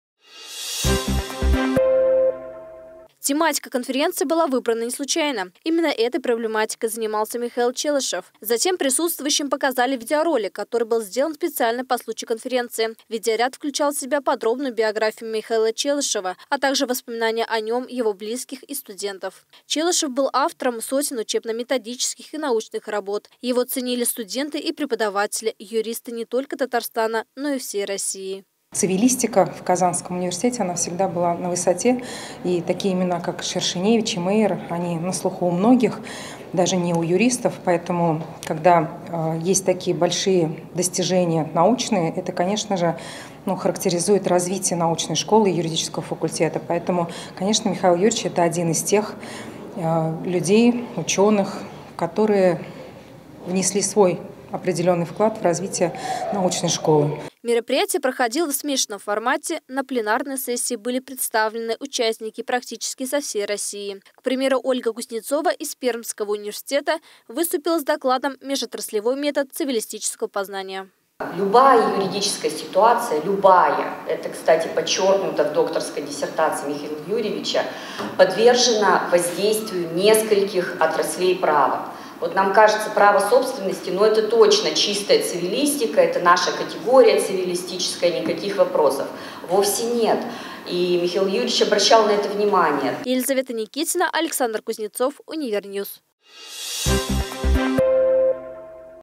КОНФЕРЕНЦИЯ, ПОСВЯЩЕННАЯ ПАМЯТИ ПРОФЕССОРА ЧЕЛЫШЕВА
Институт социально-философских наук и массовых коммуникаций КФУ уже 8-ой раз организует Садыковские чтения. На этот раз тема конференции: «XXI век: Вариативность человеческого». О всех подробностях в нашем сюжете.